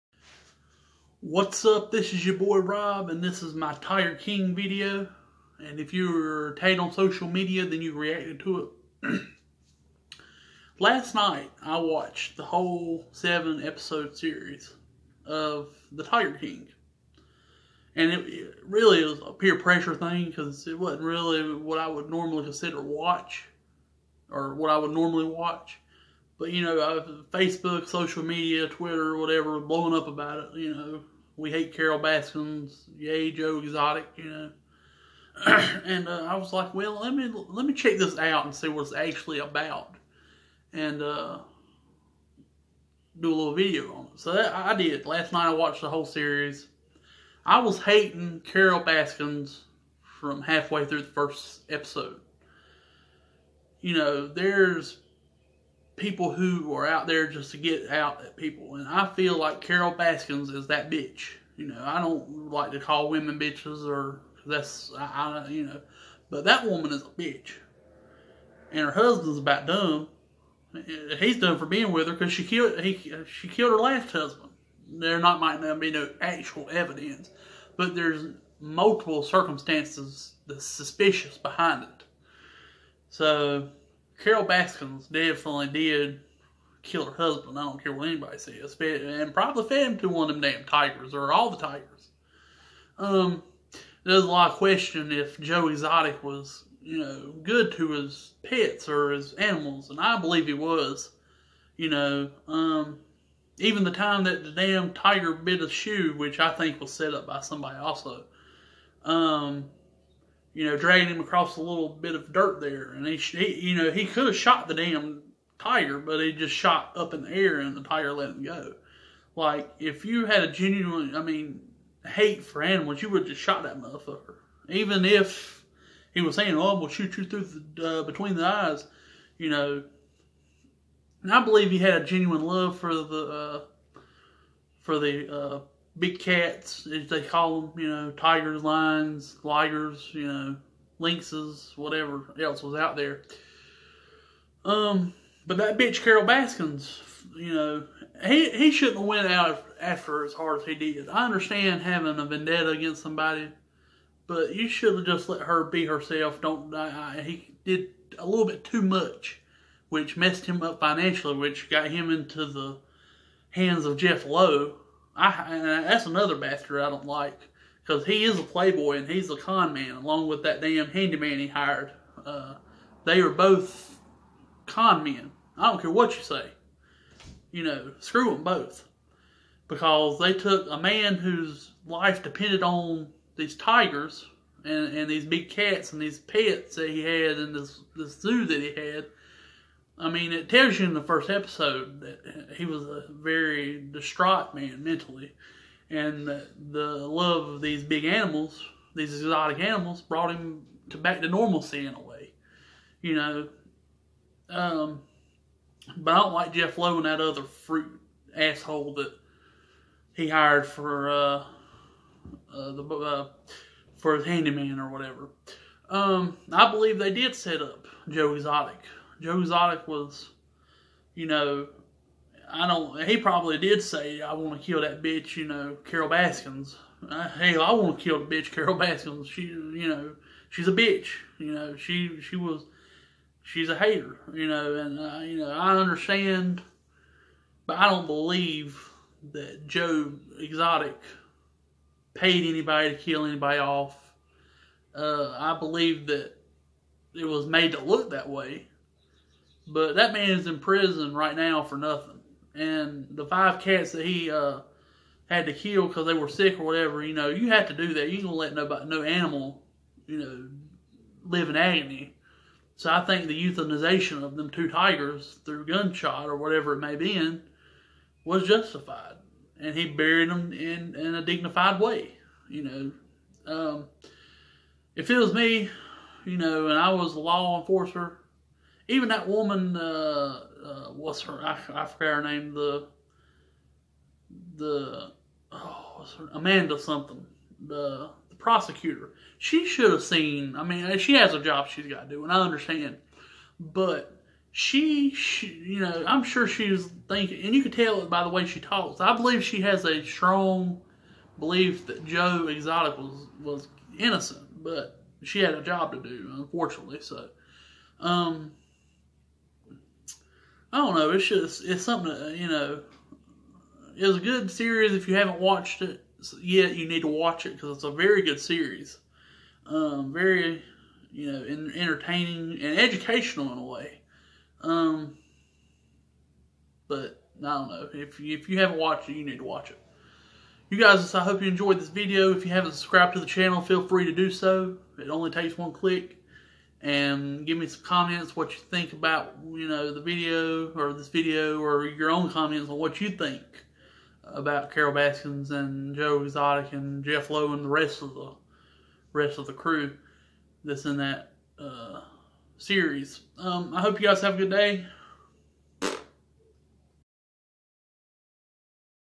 The Tiger King
A review of The Tiger King series, and some opinions